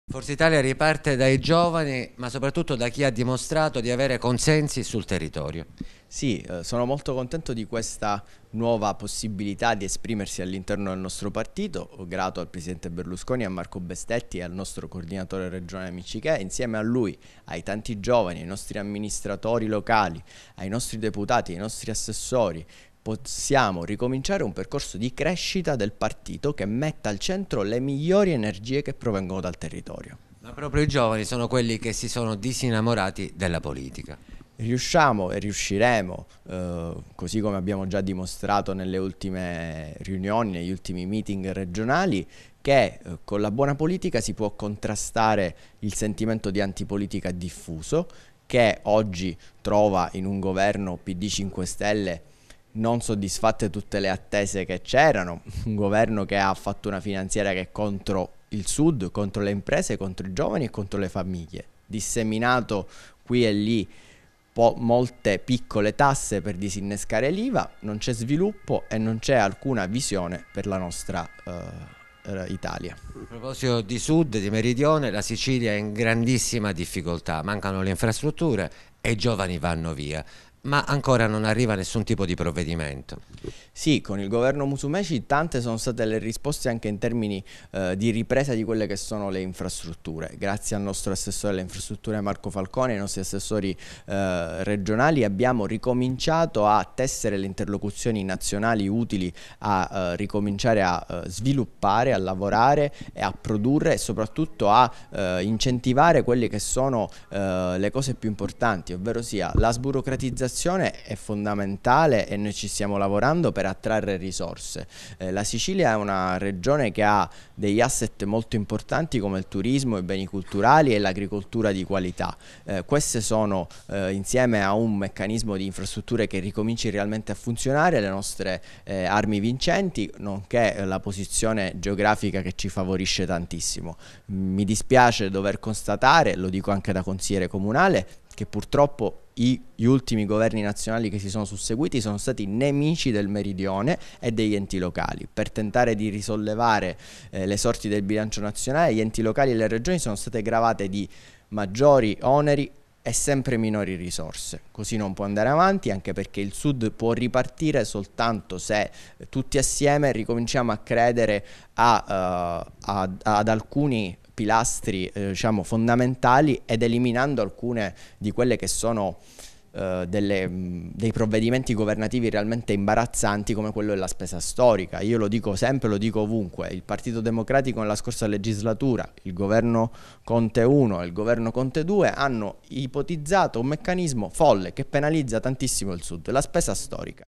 Palermo. Andrea Mineo coordinatore regionale giovani FI
Andrea Mineo, già consigliere comunale a Palermo, è stato nominato coordinatore regionale dei giovani di Forza Italia.